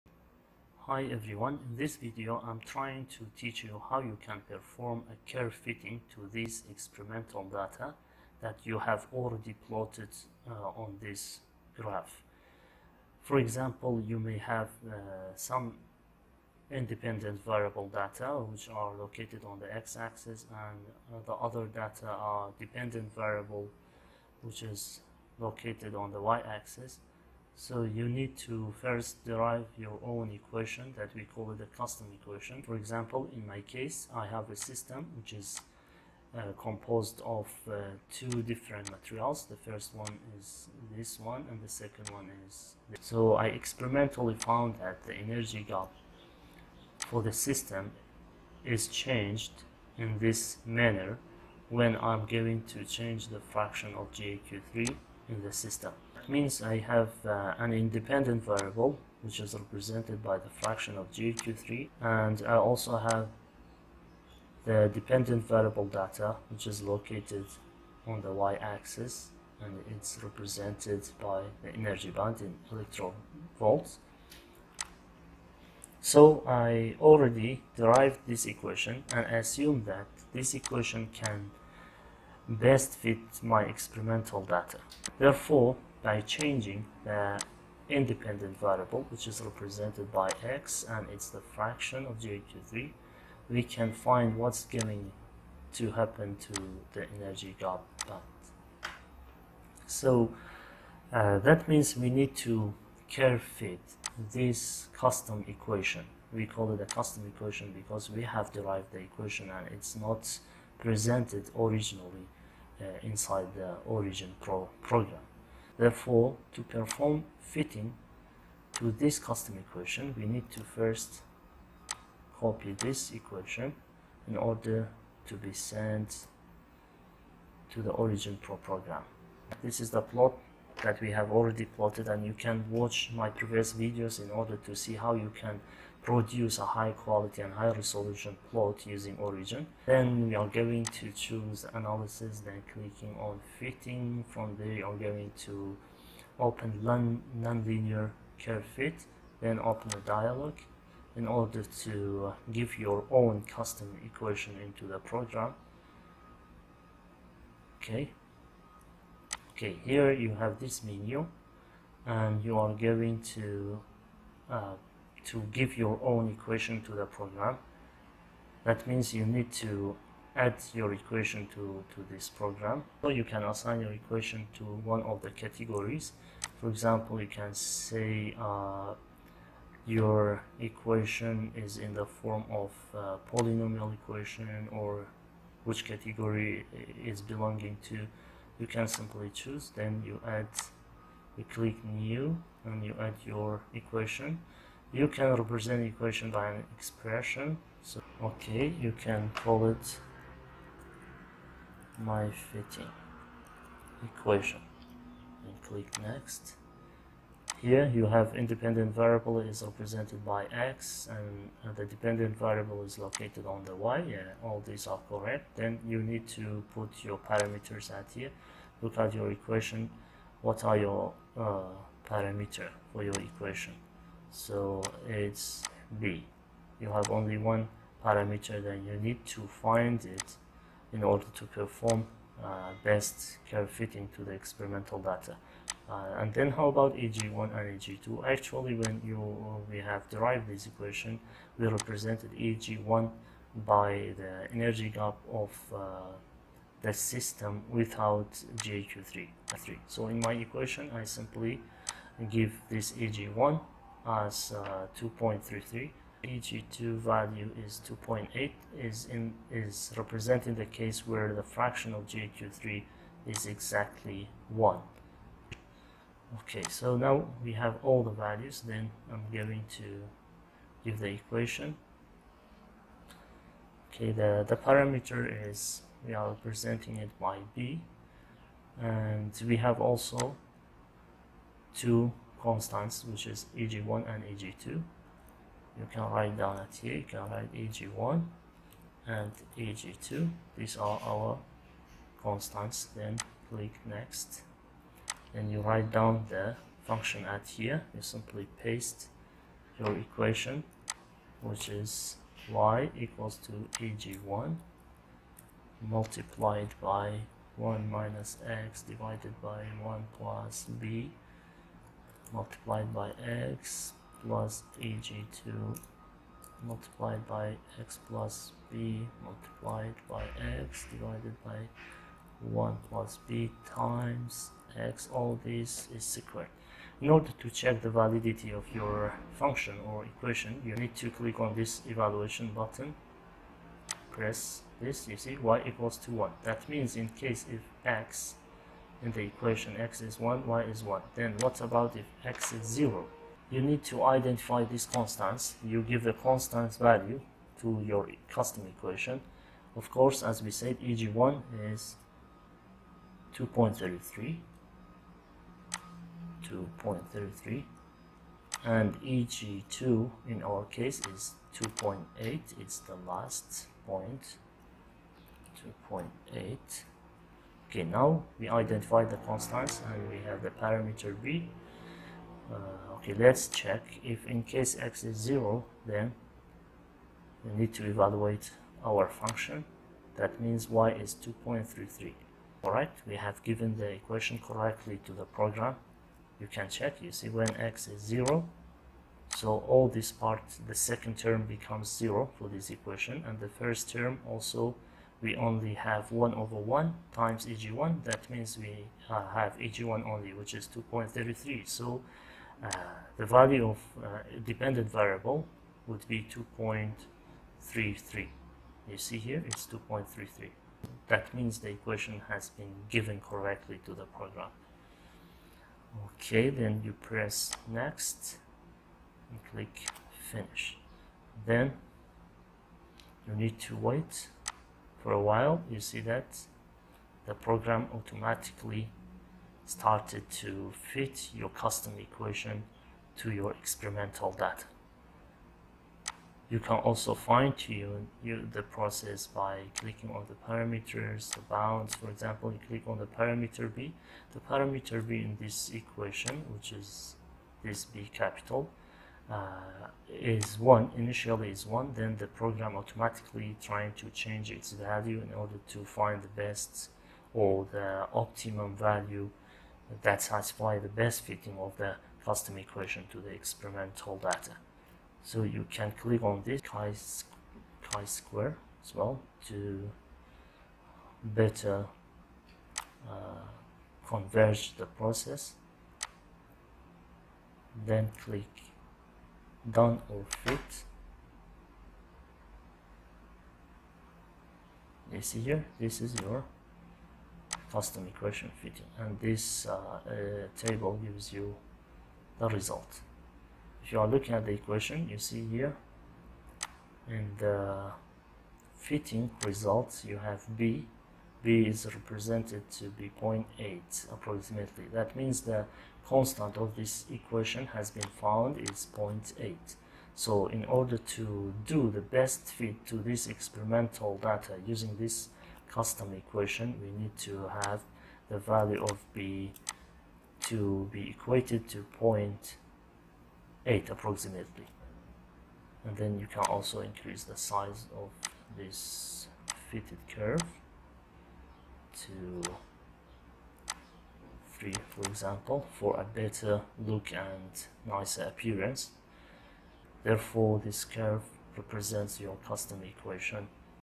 How to fit a custom equation to your experimental data using Origin pro?
In this video, a full explanation is given to show how one can perform the curve fitting of a custom equation to some experimental data using origin pro.